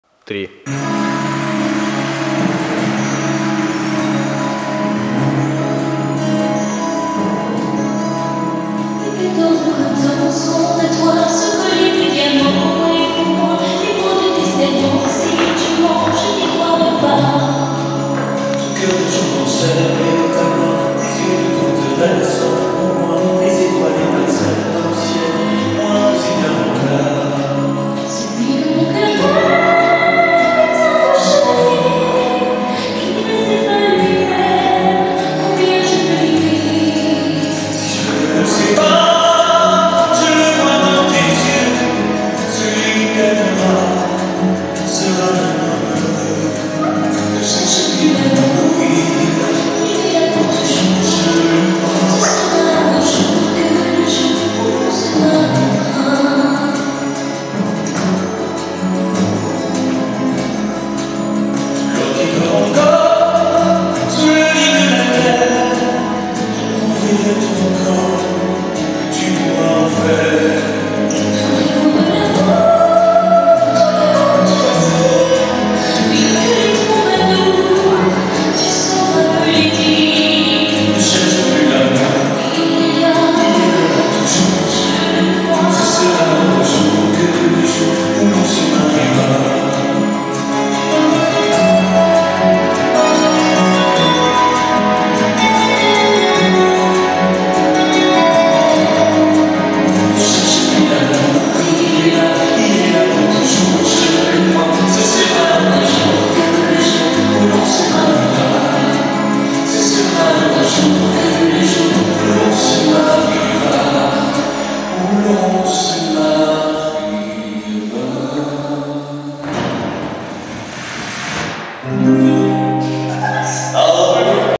Вальс
Конкурсная работа учащихся 10 и 11 классов ЧОУ "Классическая гимназия-пансион"